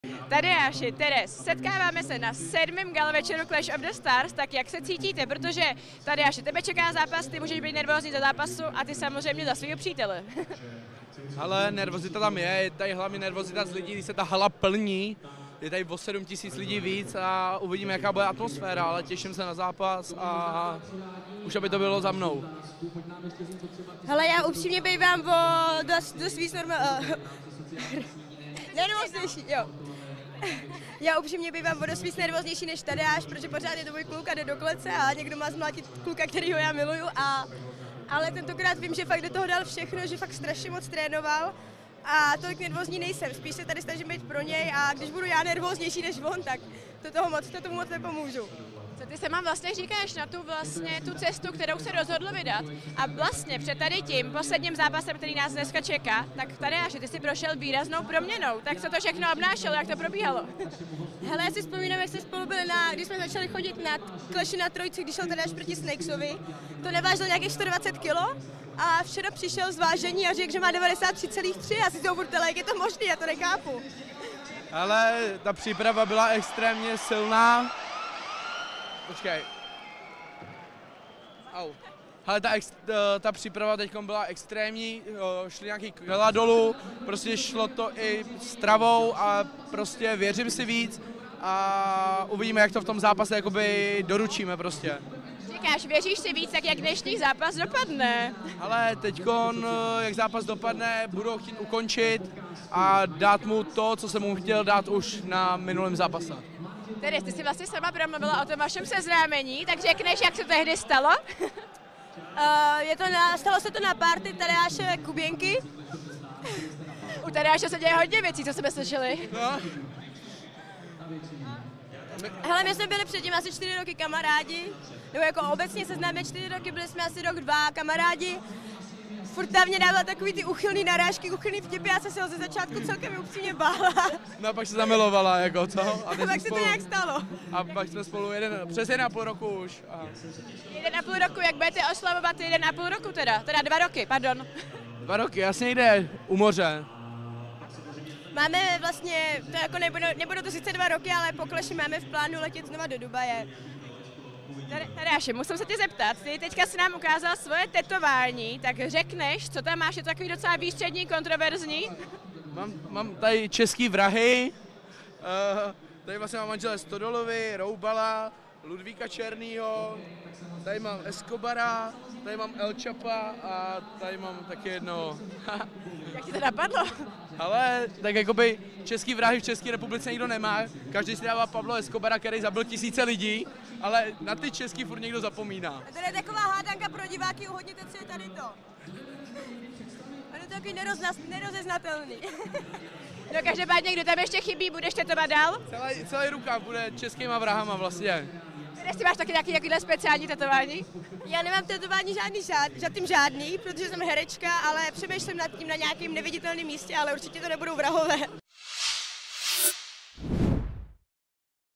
Napadený Tadeáš v Clashi: Před útokem se pochlubil tetováním s českými vrahy
Dramatická situace se odehrála během galavečera Clash of the Stars 7. Zápasník Tadeáš Veselý zrovna šel do ringu, když ho zákeřně napadl jeden z diváků. Tadeáš se po útoku skácel k zemi, zatímco útočníka pacifikovala ochranka. Redakce eXtra.cz byla celé situaci na místě přítomná a vše zaznamenala na video. Tadeáš posléze ukázal zprávu z nemocnice, kde je popsáno, že upadl do bezvědomí. Ještě před útokem se nám stačil pochlubit svými tetováními na ruce, kde má české i zahraniční vrahy.